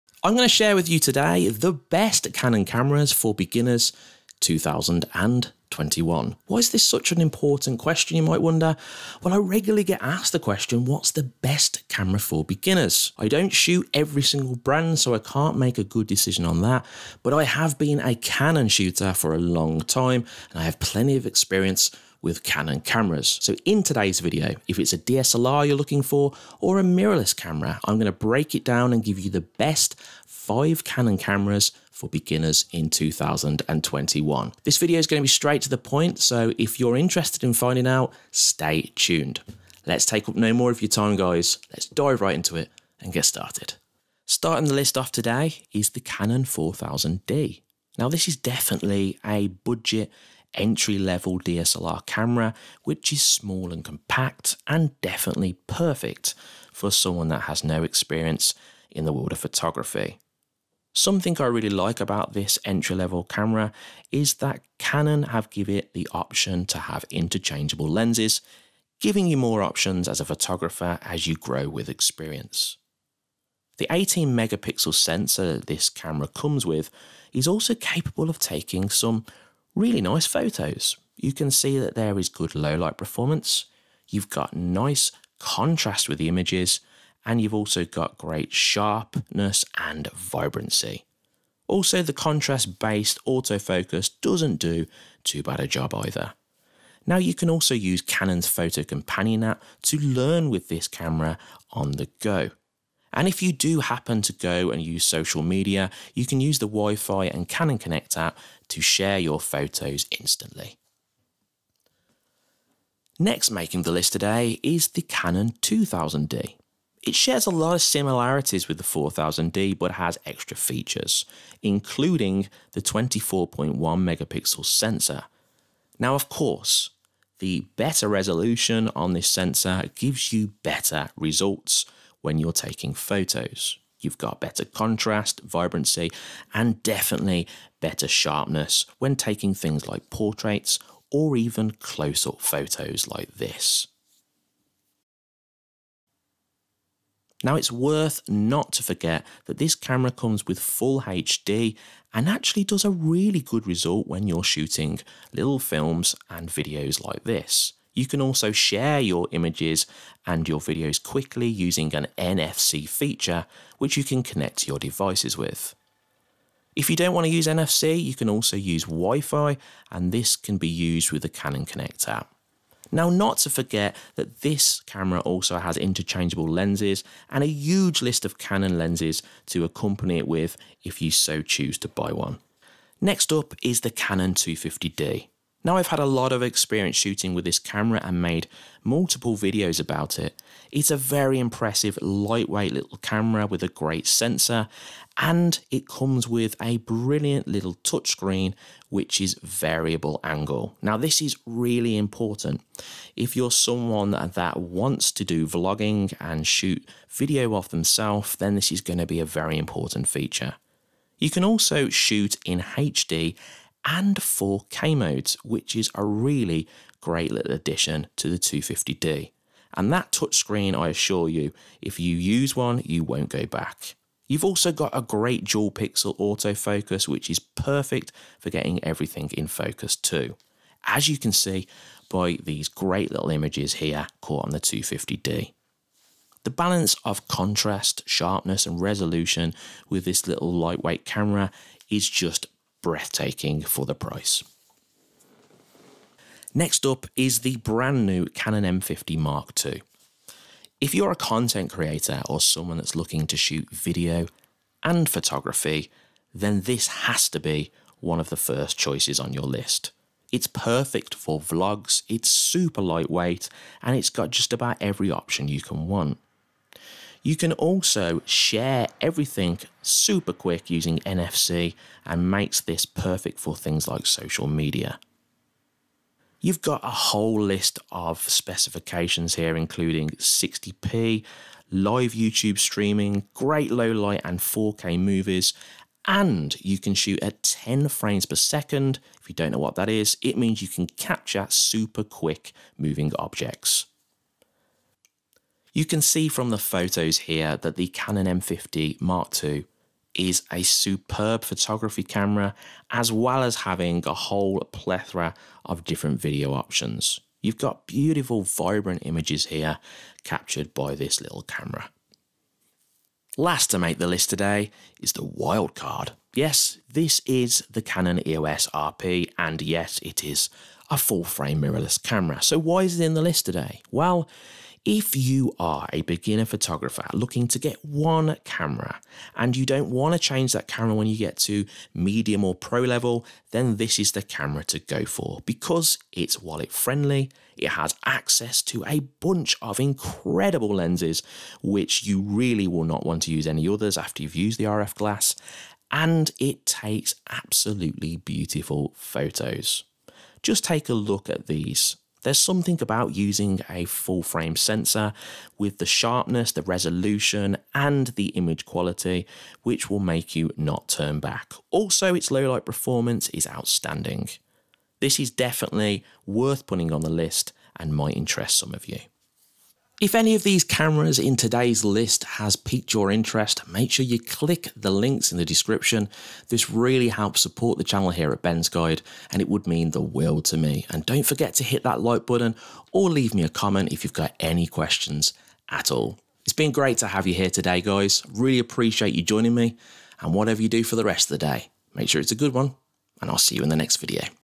Canon Cameras For Beginners in 2021
Canon Cameras For Beginners 2021, if you are photography beginner then this video will show you the best cameras you can buy from Canon in 2021.

Cameras From The Video:

✅CANON 4000D
✅CANON 2000D
✅CANON 250D
✅CANON M50 MARK ii
✅CANON EOS RP

Do you want to learn Photography, Videography or Photo editing?